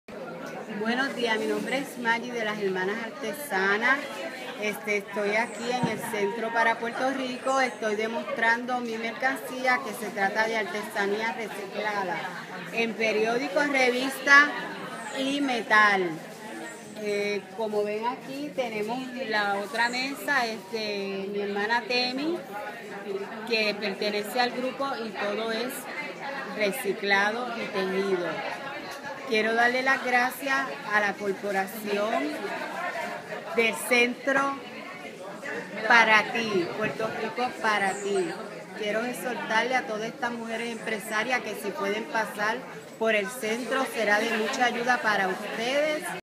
MERCADO VERDE - Centro para Puerto Rico
María Rosado García, microempresaria y artesana certificada, así como participante de nuestro Programa de Formación Empresarial para la Mujer, exhibe sus productos hechos de material reciclado durante MERCADO VERDE.

Iniciativa que el Centro para Puerto Rico ha creado para promocionar los productos agrícolas, naturales y orgánicos de las mujeres microempresarias.